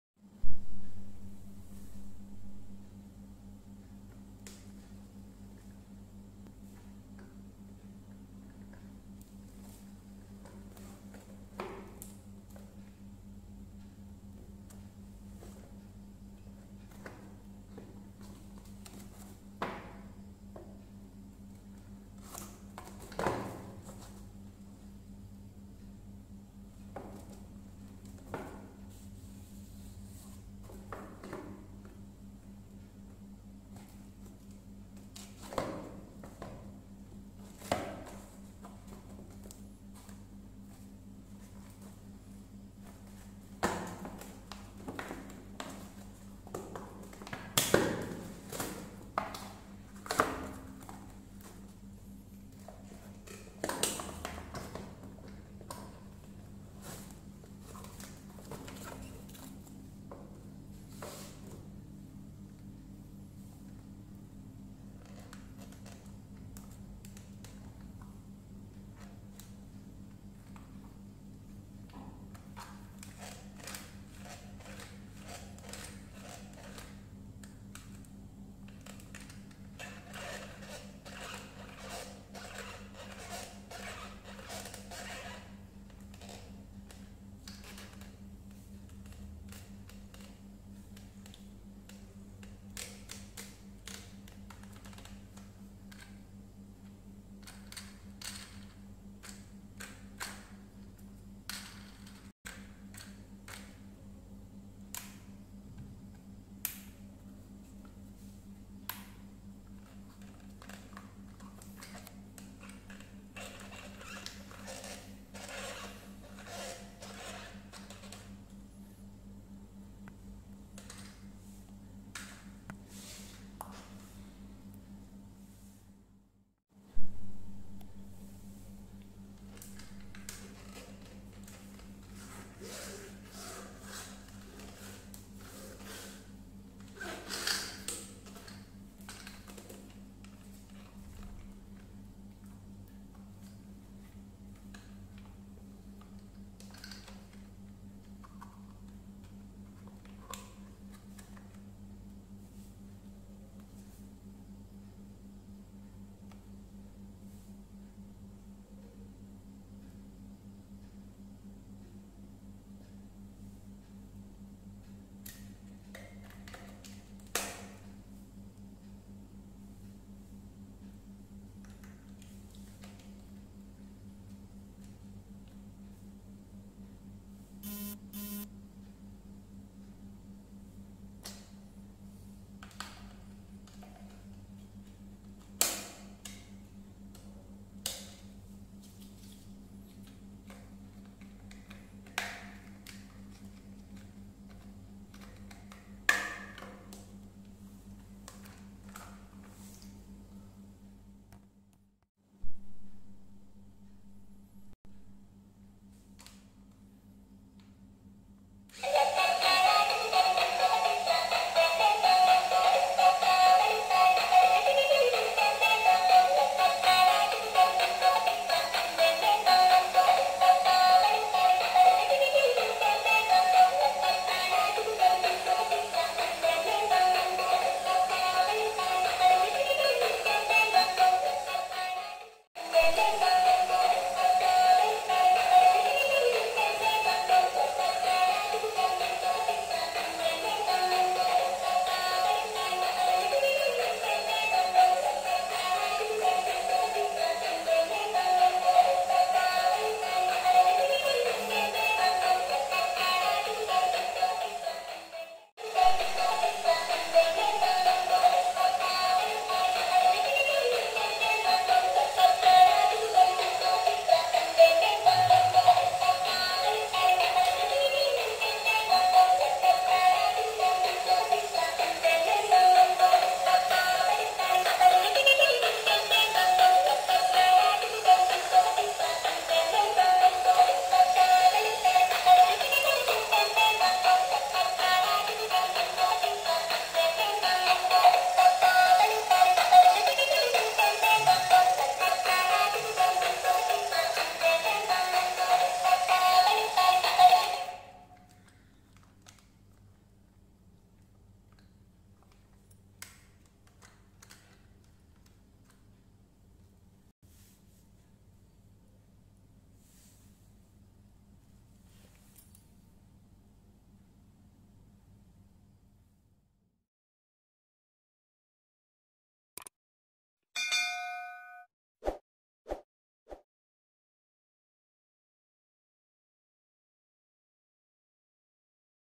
Naughty Robot | Toy Robot for Kids | Unboxing Video | Nila Art Craft and Fun
Here we unboxing a naughty robot for you. He can sing and dance.